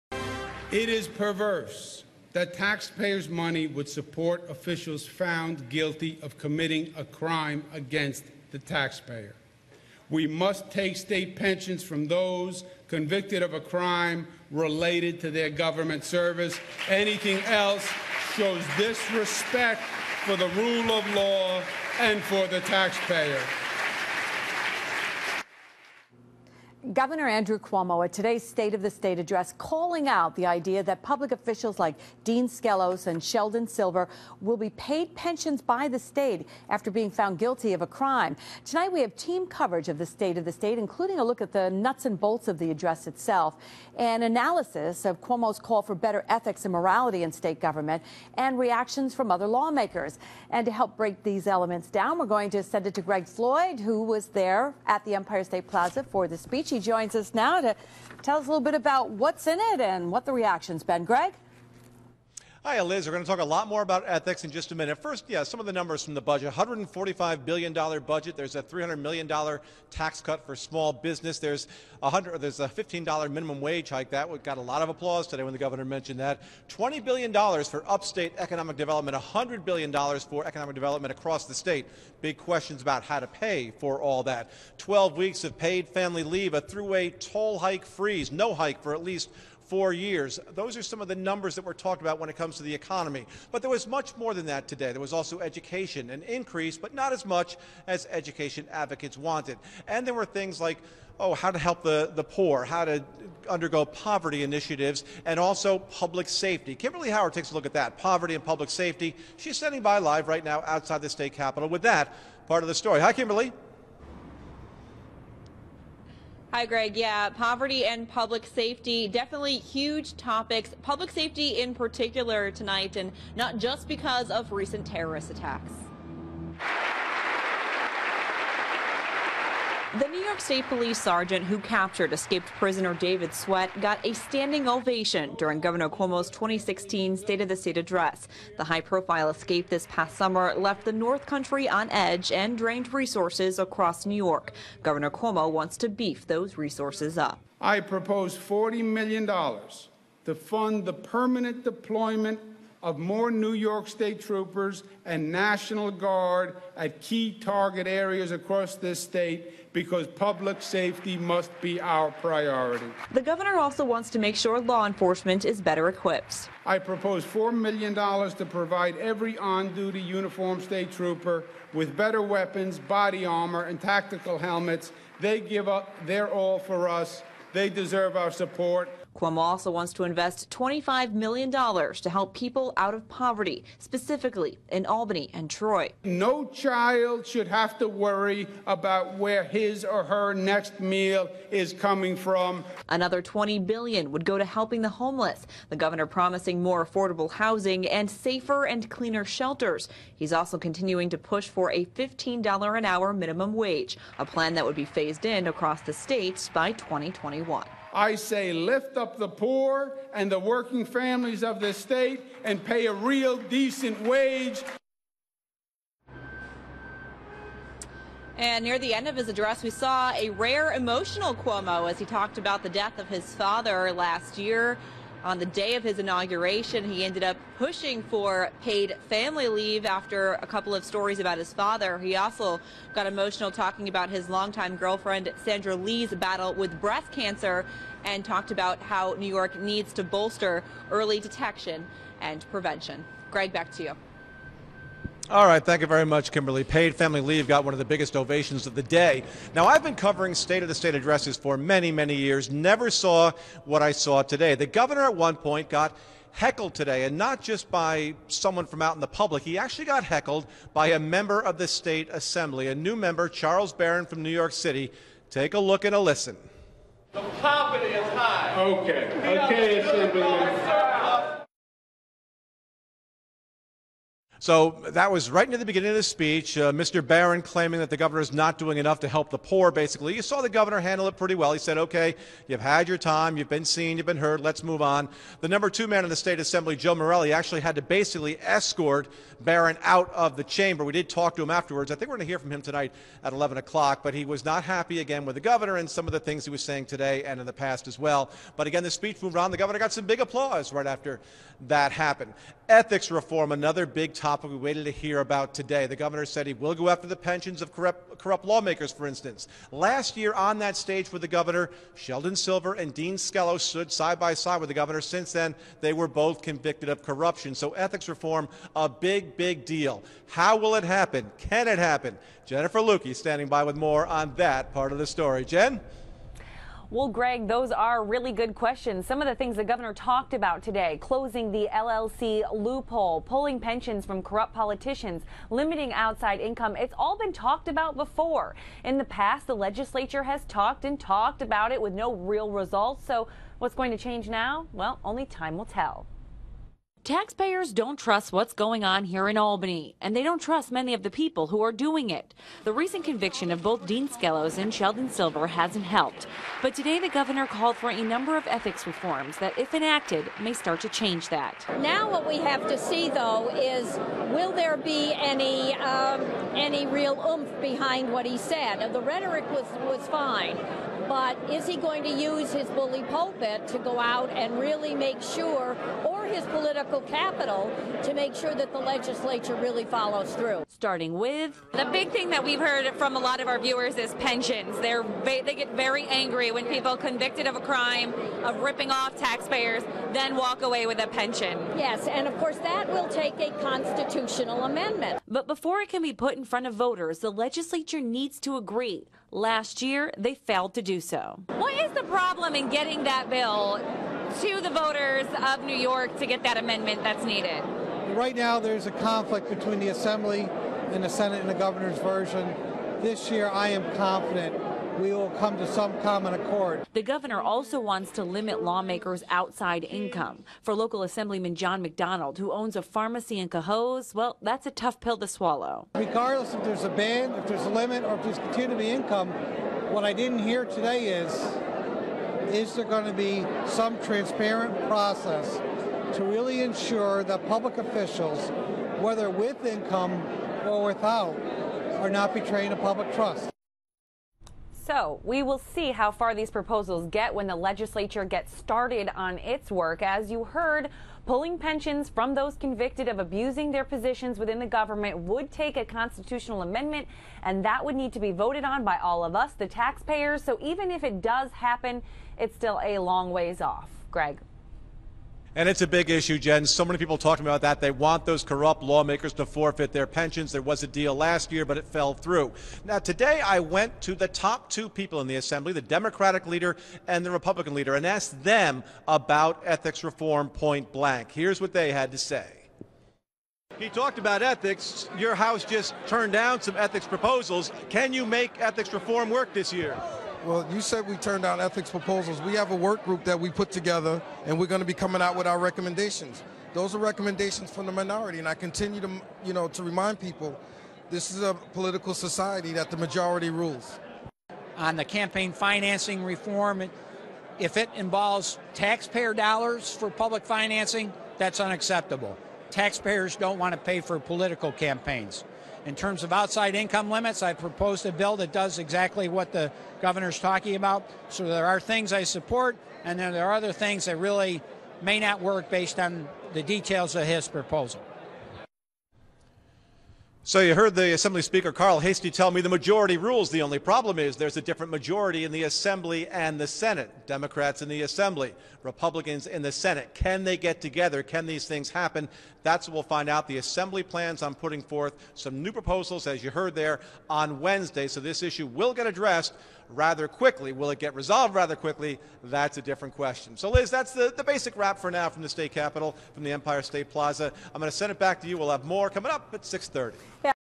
2016 State of the State: Poverty and Public Safety
ALBANY -- CBS6 brings you team coverage of Governor Cuomo's 2016 State of the State address.